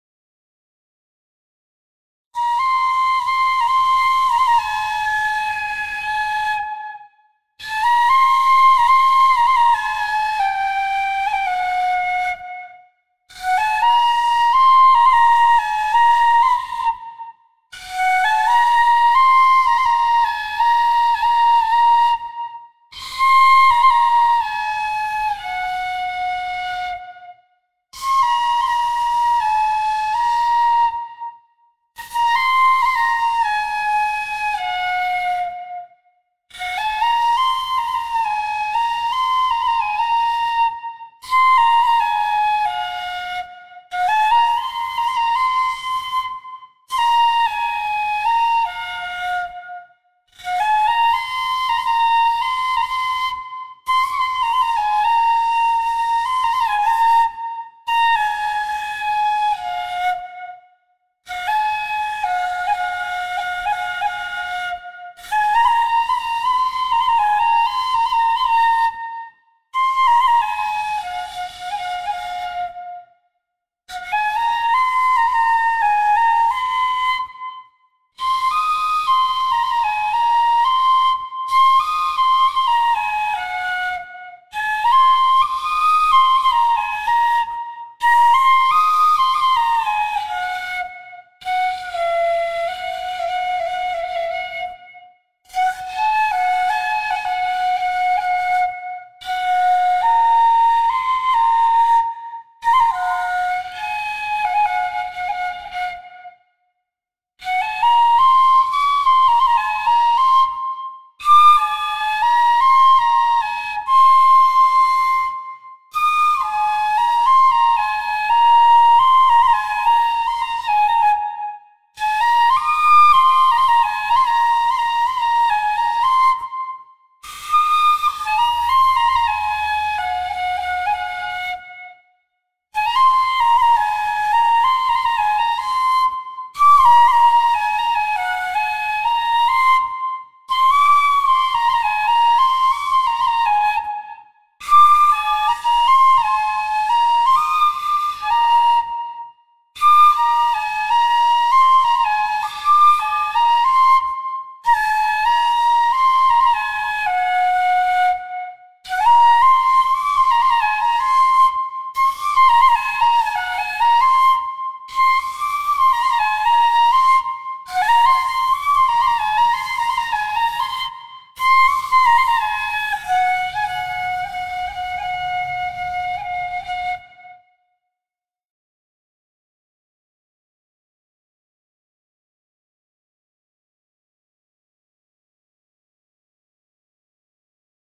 篠笛 7 - Shinobue Flute
I enjoy playing the Shinbone Flute. It is my one of me favorite instruments.